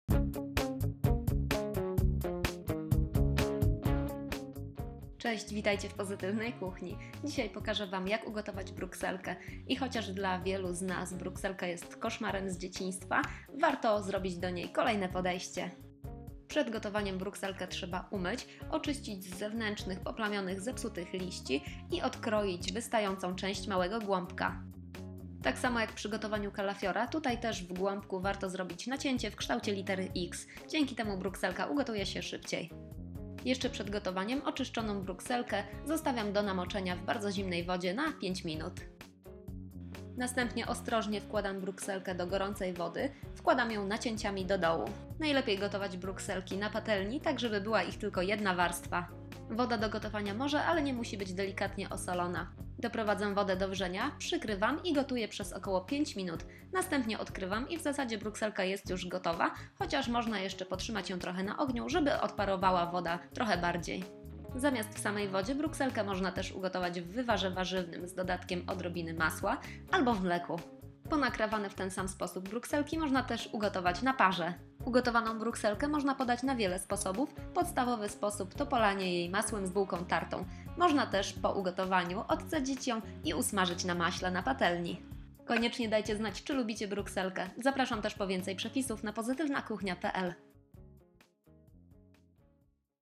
Jak ugotować brukselkę? 🥬 Gotowana brukselka | Dorota Kamińska
Jak ugotować brukselkę? Gotowana brukselka krok po kroku. Przepis na gotowaną brukselkę, błyskawiczny :). Brukselkę można gotować w wodzie albo w wywarze warzywnym lub drobiowym. Można też wrzucić ją do mleka albo ugotować na parze. Po ugotowaniu brukselkę polewam bułką tartą z masłem albo smażę na maśle na patelni.

Na skróty:
00:00 Intro
00:15 Mycie i oczyszczanie z zewnętrznych liści
00:24 Nacinanie spodu w kształcie litery "X"
00:32 Moczenie w bardzo zimnej wodzie przez 5 minut
00:41 Wkładanie do gorącej wody, nacięciem do dołu
00:53 Gotowanie przez około 5 minut
00:57 Odkrywanie
01:00 Inny sposób: gotowanie na parze, w wywarze warzywnym lub mleku
01:18 Brukselka z bułką tartą z masłem
01:27 Zakończenie